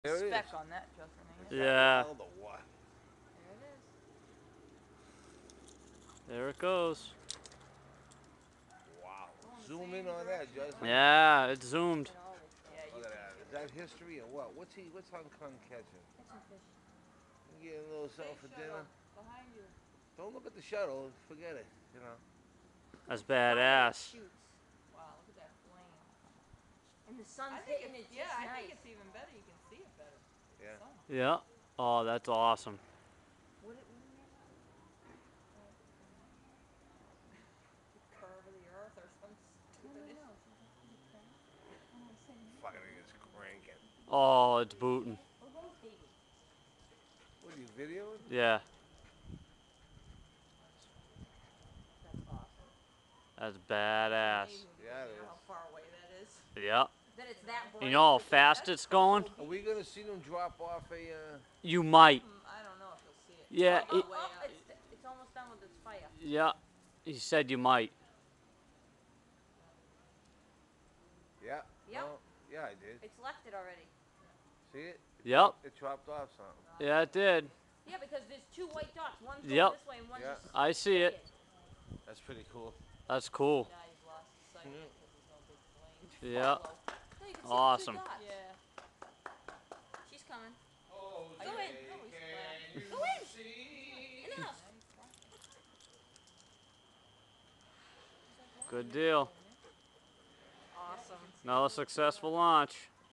Space shuttle launch 2009
video of the space shuttle launch from my florida house